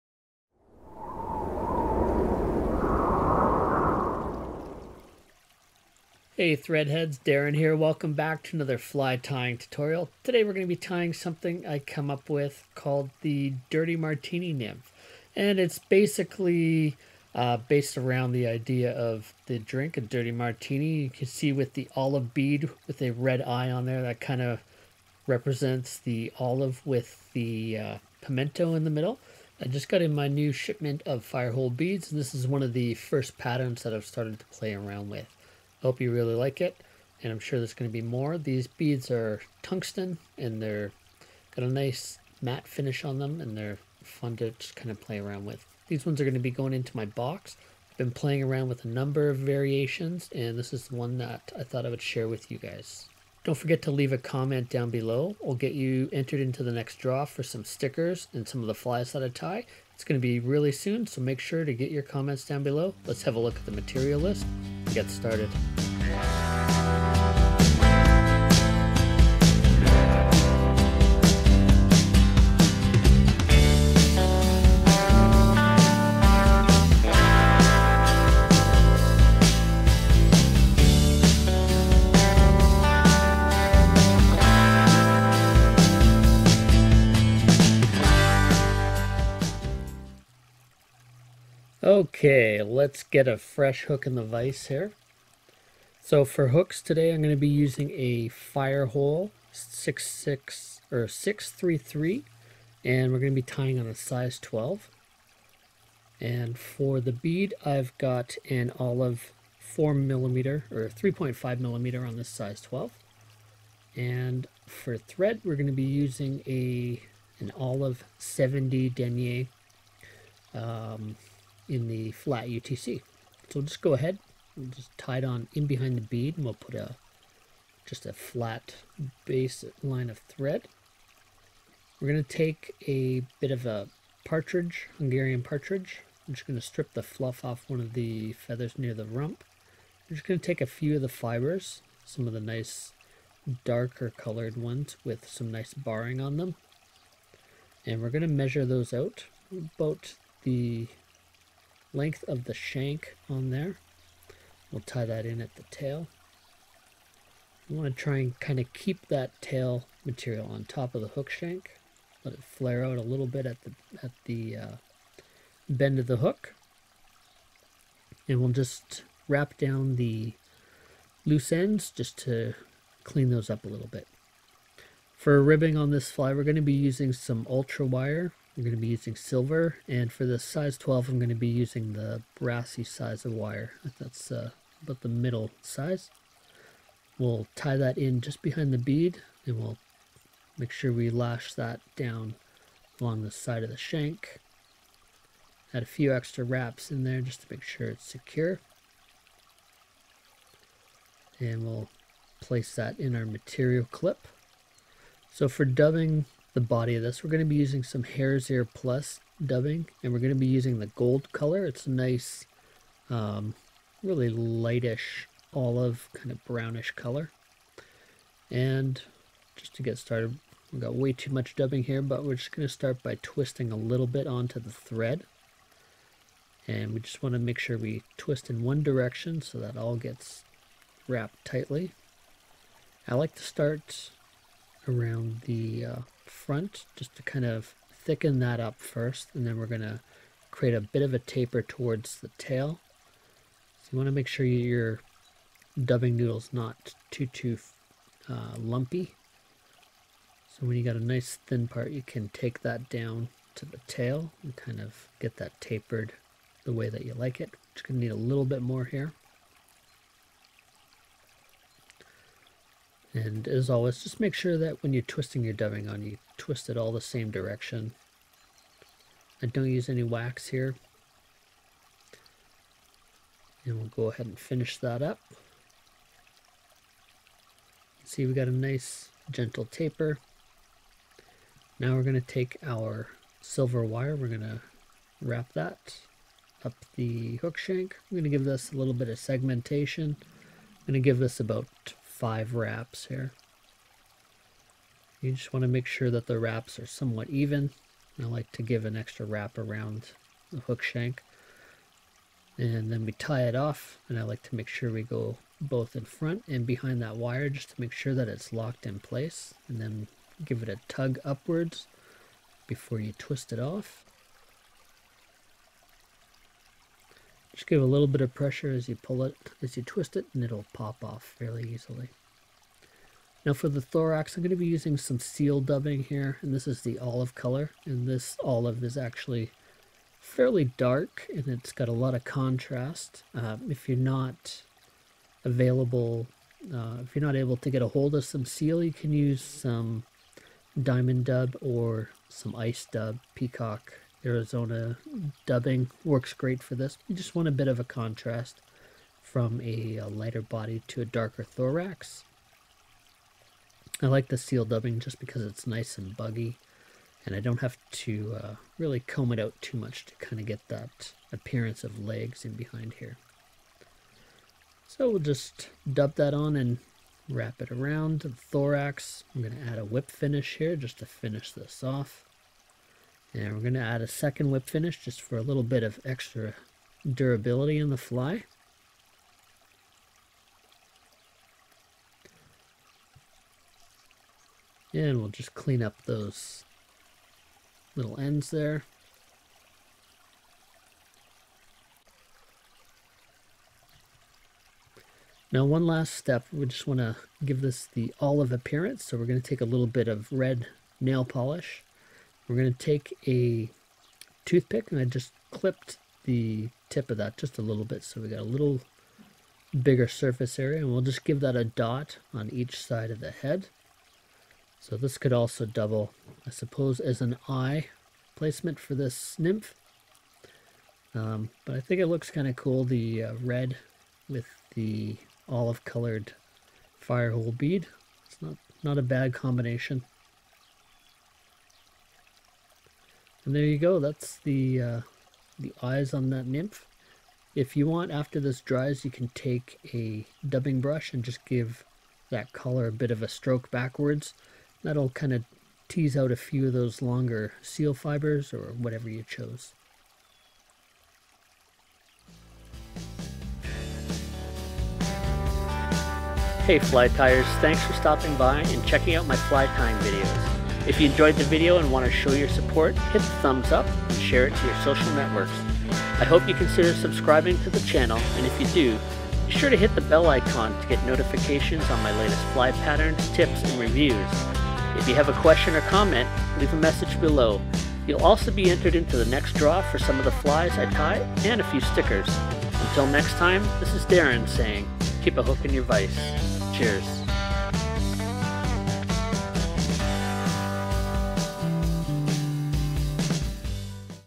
Tying a Dirty Martini Nymph Fly pattern for trout - Ep147 PF
Hey Thread Heads, I just received my order of Firehole stones in the mail. I've been anxious to get these as I had a few patterns I was wanting to tie for the channel. The Dirty Martini nymph is modeled after the cocktail and the olive bead with its red eyes mimic the olive often found swimming in the glass.

I've taken this pattern through a number of different versions before hitting on this configuration. The seal dubbing is great for a natural collar and it can be brushed out to mimic legs. If you do not have access to seal, another dubbing like Simi-seal, Antron, or ice dubbing should fill in fine.

Dirty Martini Nymph

Hook: Firehole 633 #10-18
Bead: Firehole Tungsten Olive
Thread: Olive 8/0 (70d)
Tail: Hungarian partridge fibers
Rib: Silver wire
Body: Gold Hare’s Ear Plus Dubbing
Thorax: Olive seal dubbing or similar
Eyes: Red paint (nail polish)

Camera: Nikon CoolPix B700
Vise: Griffin Montana Mongoose